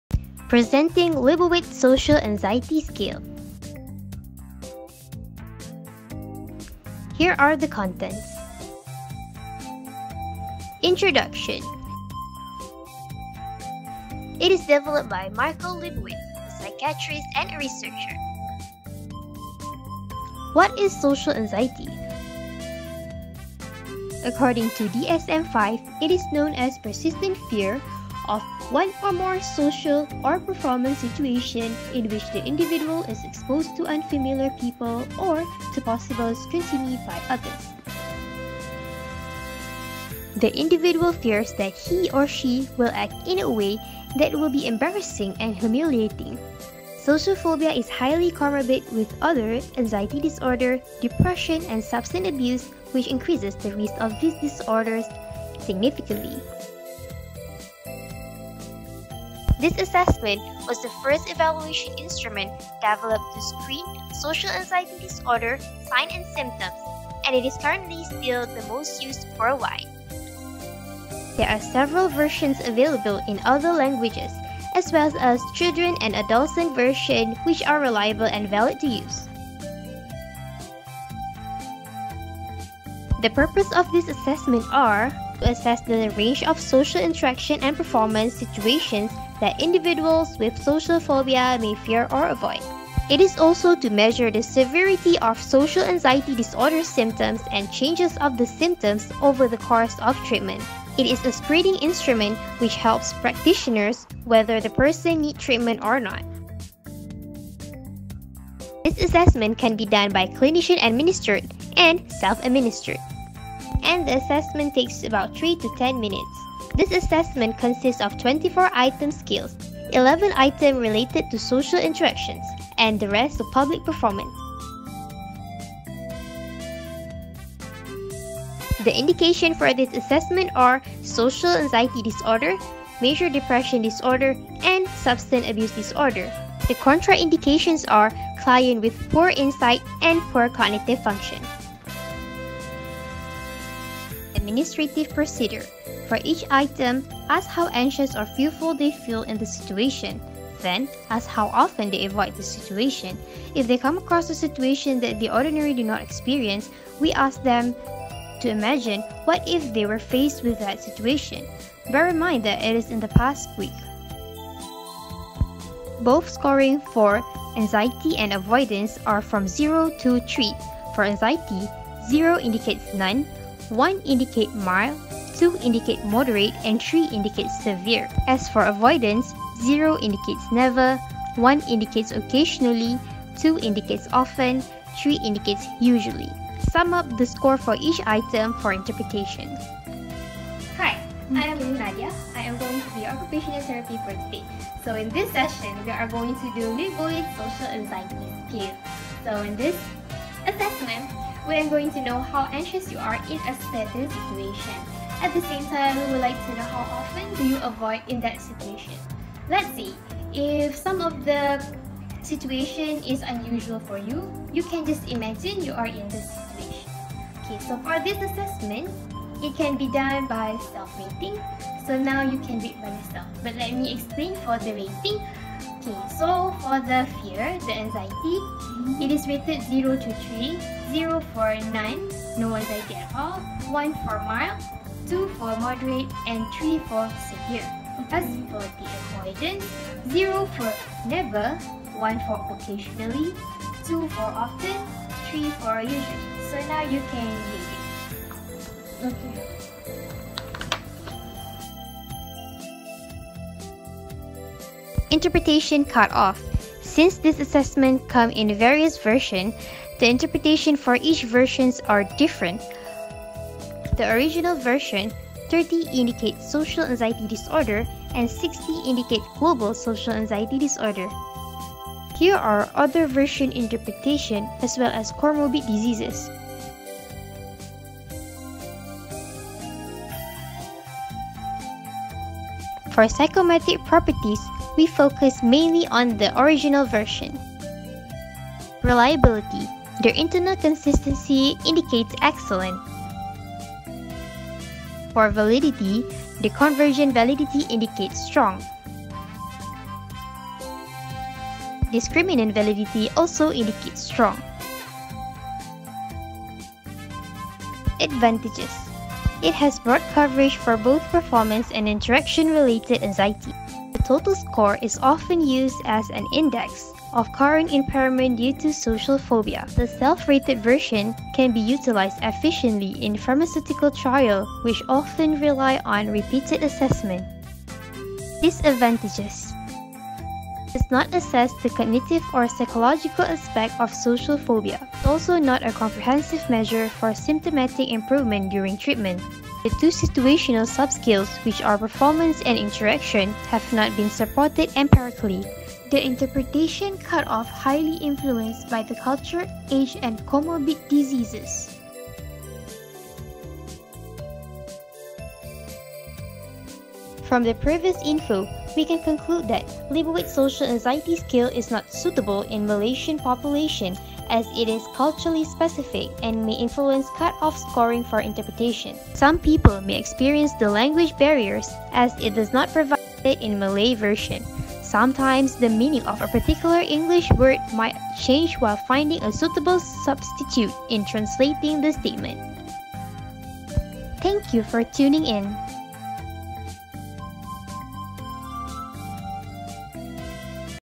Lesson 7: Liebowitz Social Anxiety Scale
At the end of this video, you should be able to know the developer, background, purpose, type, components being assessed, target population, administration procedure, scoring interpretation, psychometric properties, advantages, and disadvantages of  this assessment.

A simple activity for you to undertake after watching the video as follow: